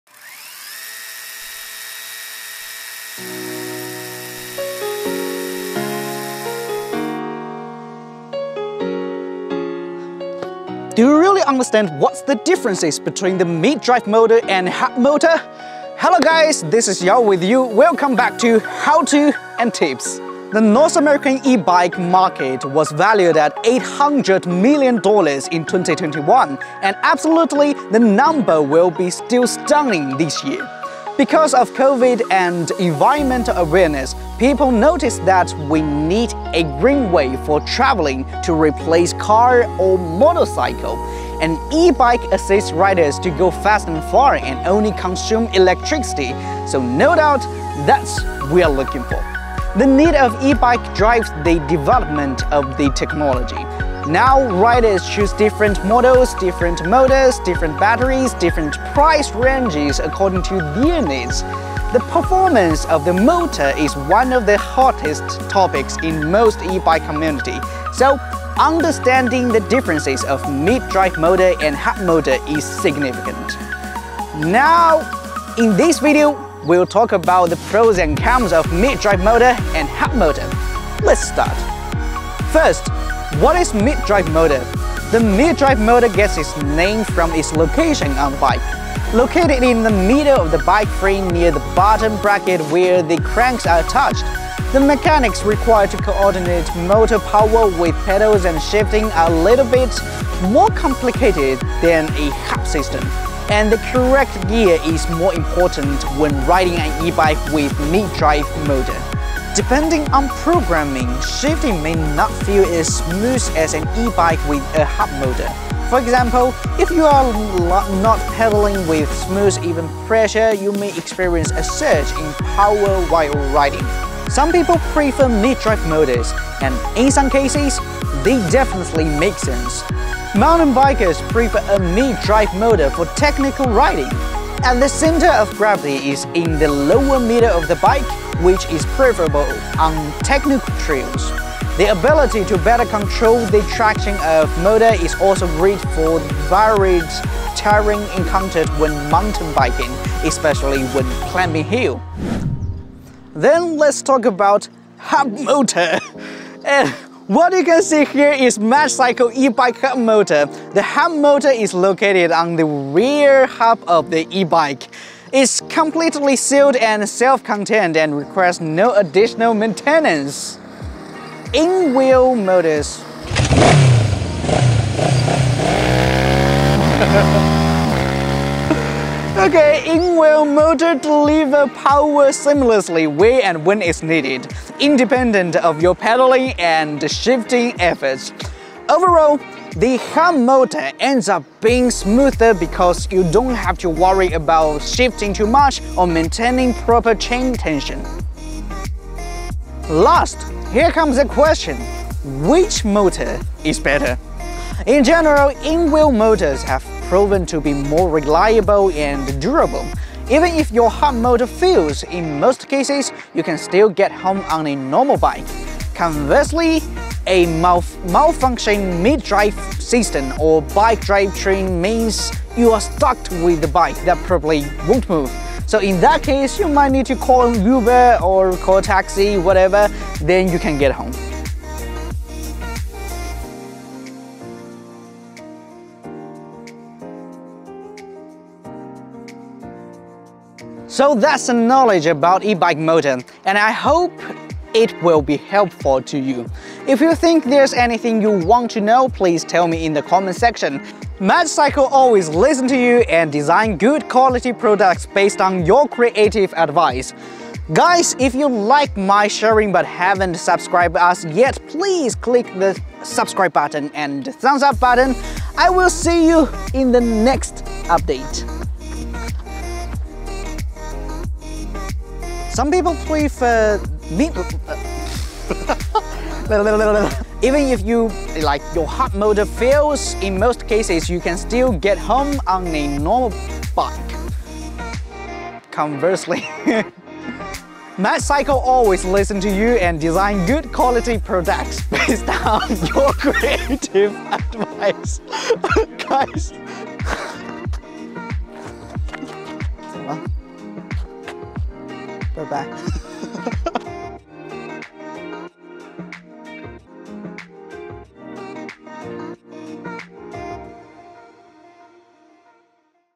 Mid Drive? Hub motor? What you Should Know about Ebike Motors
Before you jump to take your Magicycle home, what you should know about ebike motors. Here are the facts.
What's mid drive motor and what's hub motor?
Which one should you choose?